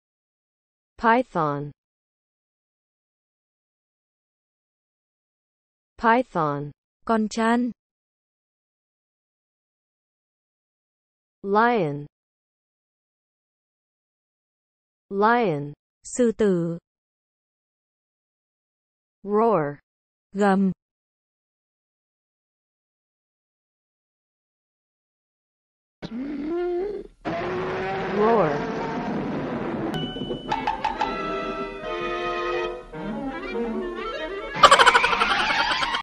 3. python - lion # Học tiếng anh
✍️ Học tiếng anh, các từ:
python: con trăn;  lion: sư tử;  roar: gầm, rống
🚩 Học ít nhớ kỹ